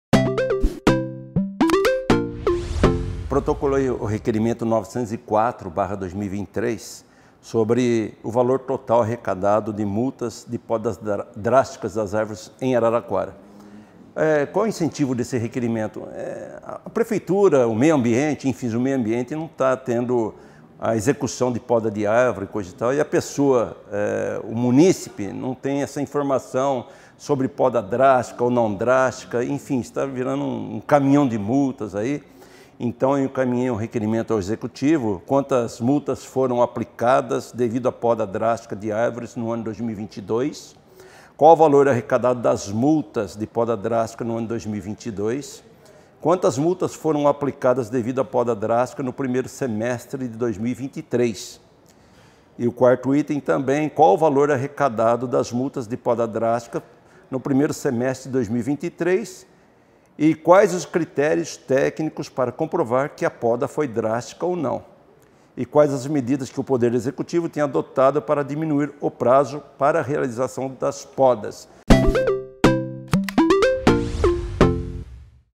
Fala, Vereador! – Lucas Grecco: Multas podas drásticas
Confira fala do vereador Lucas Grecco sobre o requerimento de sua autoria, em que pede ao Município informações das multas emitidas devido à podas drásticas como, quantidades, valores arrecadados, critérios para aplicação, entre outras.